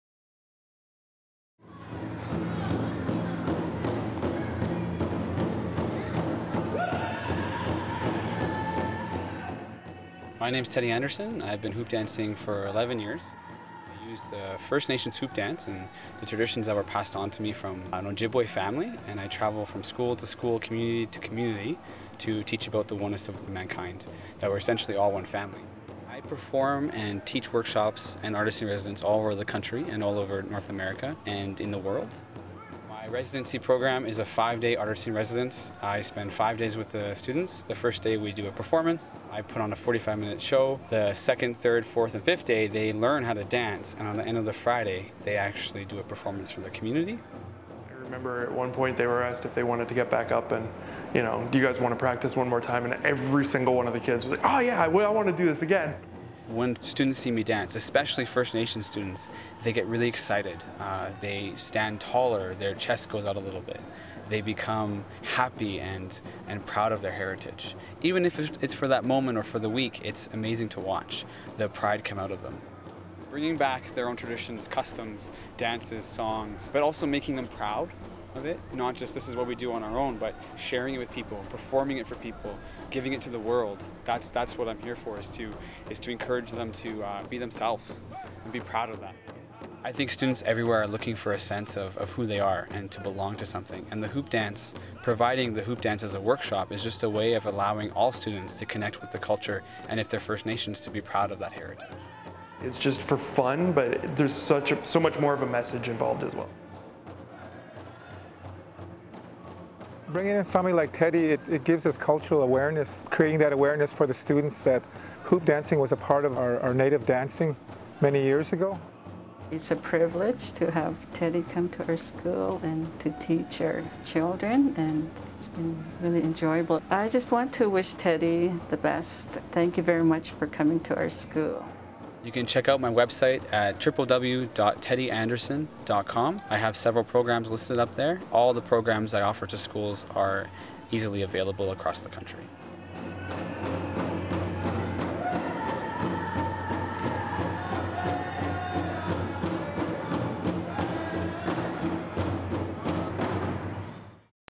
Hoop Dancer Teddy Anderson Artist in Residency Program
Teddy Anderson is a World Renown Hoop Dancer that has performed in 20 countries in the World. Teddy dances with up to 30 hoops to share a message of Peace, Love and Inclusion. Teddy offer performance, workshops and week long residencies where Teddy teaches students how to dance.